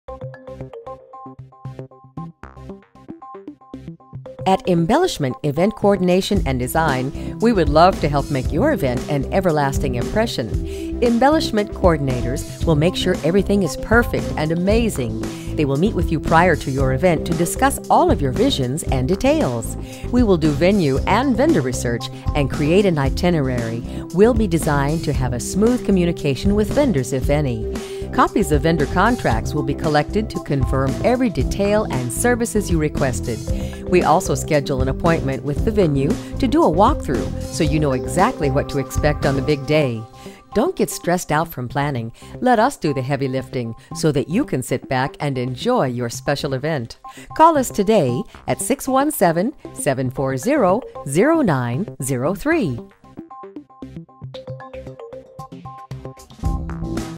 Embelish It! Event Coordination & Design Video | Events in Dorchester
Embellish It! coordinators will make sure everything is perfect and amazing! They will meet with you prior to your even to discuss all of your visions and details. We will do venue and vendor research and create an itinerary will be designed to have a smooth communication with vendors if any. Copies of vendor contracts will be collected to confirm every detail and services you requested.

12506447

Event, Wedding, Dorchester